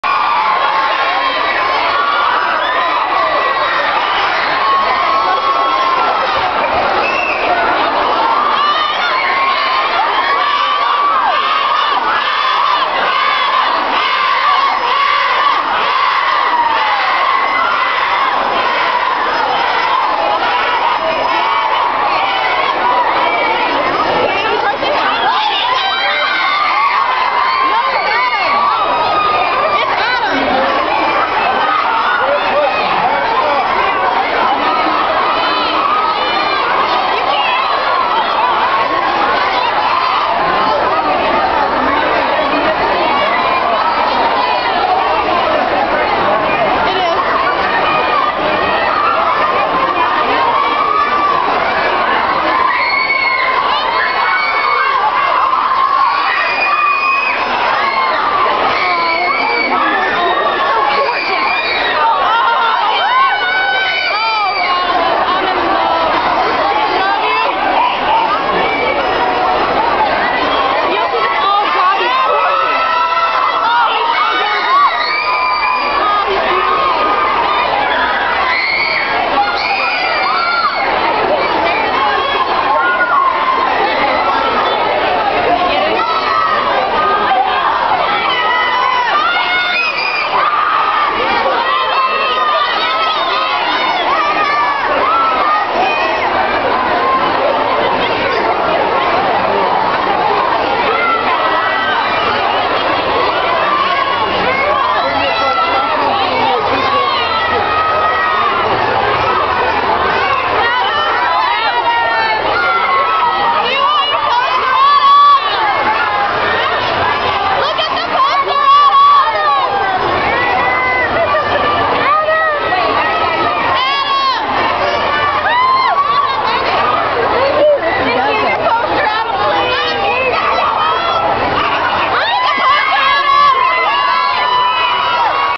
Adam Lambert- Meet and Greet Tulsa, OK (Part 2)
This was after the Tulsa show and Adam was the first one out! I love Adam and made him a big glittery poster :) at about 2:12 he finally sees it and points!!  If you take the time to leave a comment thank you and please be nice :) Thank you guys!!!